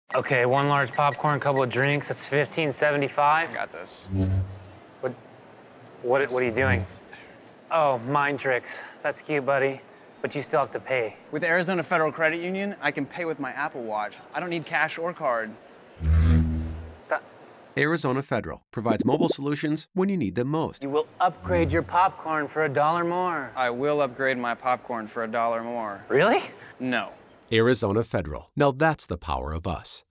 Arizona Federal Apple Watch Commercial
At Arizona Federal Credit Union, we're not just a local, Phoenix-based cooperative, we're leading the way in cutting-edge technology to manage your finances.

Our members have access to easy log in solutions, like TouchID and EyePrint ID, to an Apple Watch version of our app and even a remote control for their Arizona Federal credit and debit cards.

To learn more about membership, visit ArizonaFederal.org/whyus